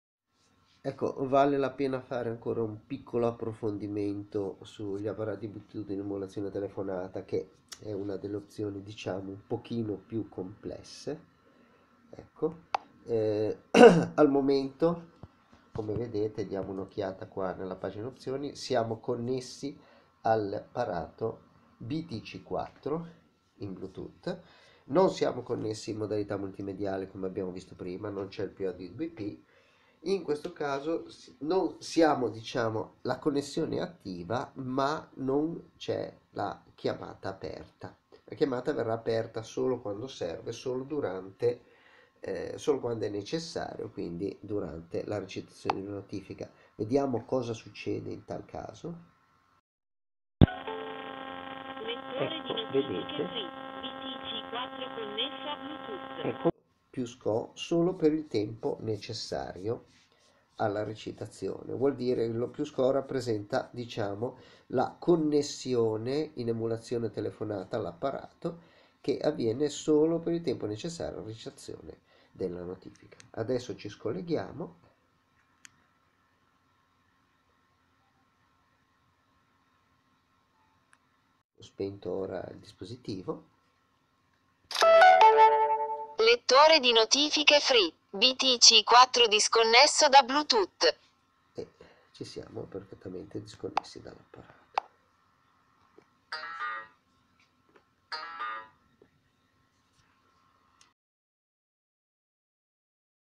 E2.3. BT Emulazione telefonata parte II (AUTORADIO)
Una seconda sezione descrittiva che aiuta a comprendere il tema, non semplice, della connessione a dispositivi bluetooth che possono essere ingaggiato solo in EMULAZIONE DI TELEFONATA, come ad esempio le AUTORADIO o vecchi auricolari bluetooth che supportano solo telefonate.